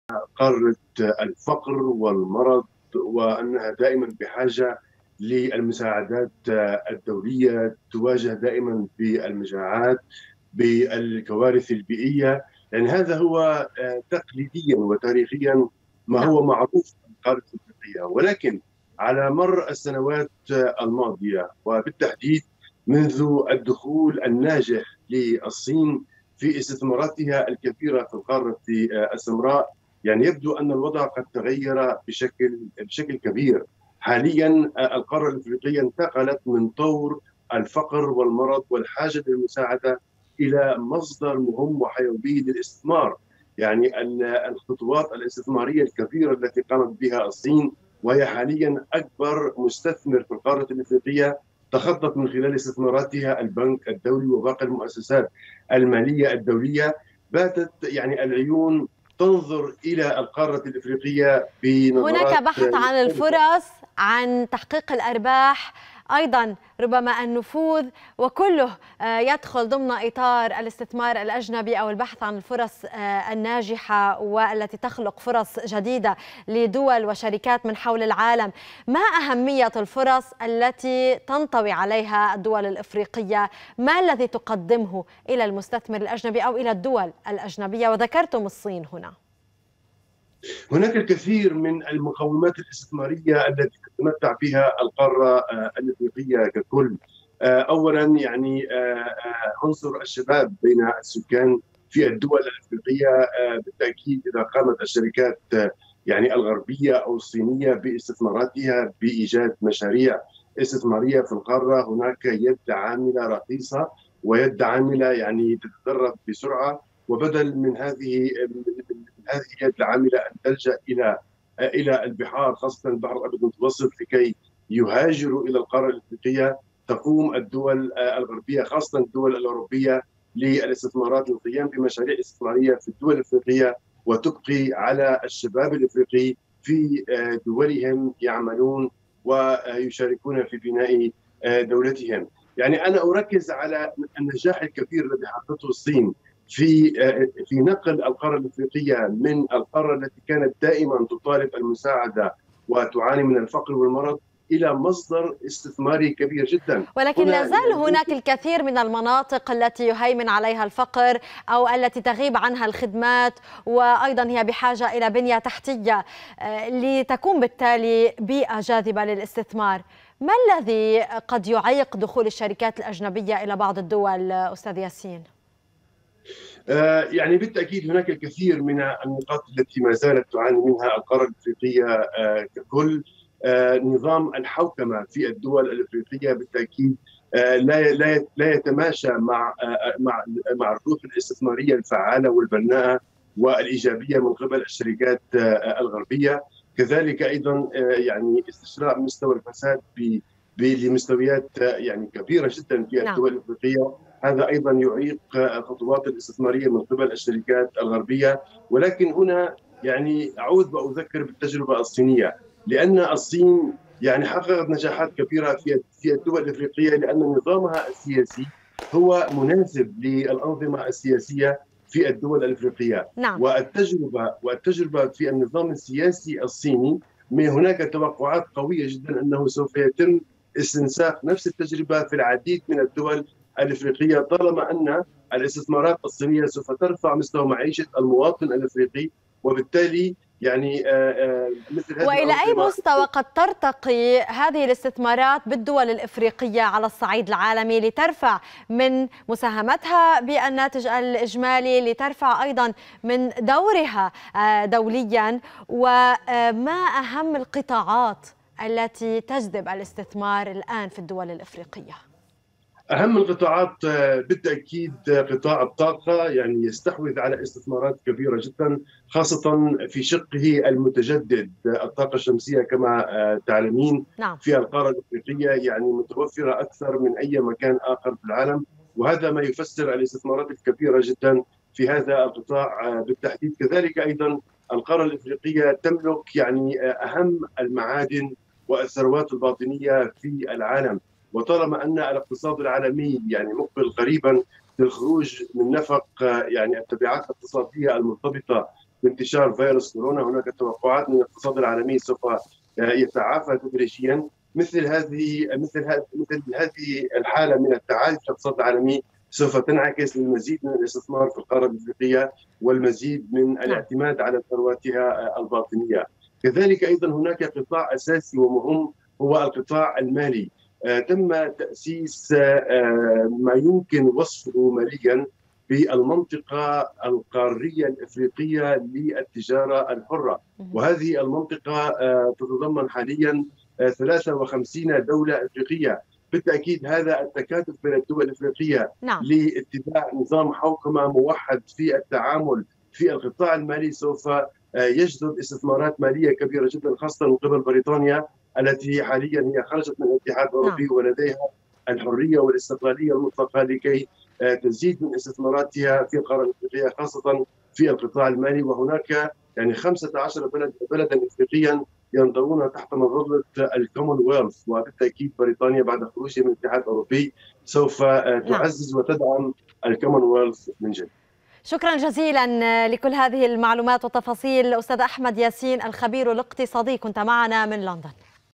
الخبير الاقتصادي أحمد ياسين: الصين أظهرت جاذبية إفريقيا كوجهة استثمارية
وفي لقاء مع سكاي نيوز عربية أكد الخبير الاقتصادي أحمد ياسين على أهمية الدور الذي لعبته الصين في إبراز إفريقيا كوجهة استثمارية هامة